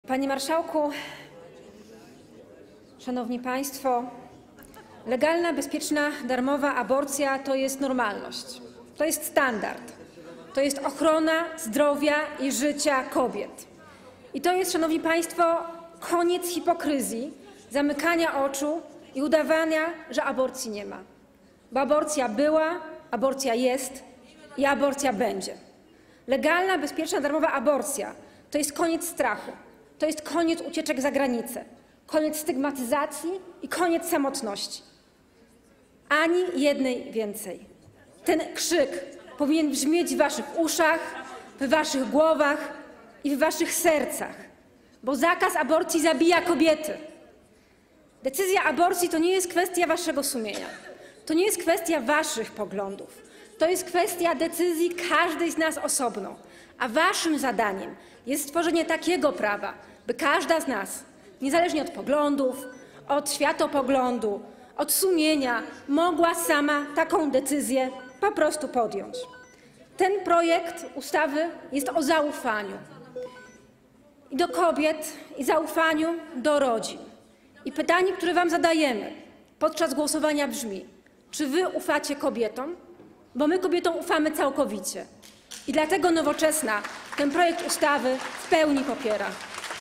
Monika Rosa - Wystąpienie z dnia 22/23 czerwca 2022 roku z godziny 0:21
Pierwsze czytanie obywatelskiego projektu ustawy o bezpiecznym przerywaniu ciąży i innych prawach reprodukcyjnych (druk nr 2298) (5-minutowe oświadczenia w imieniu klubów i 3-minutowe oświadczenia w imieniu kół)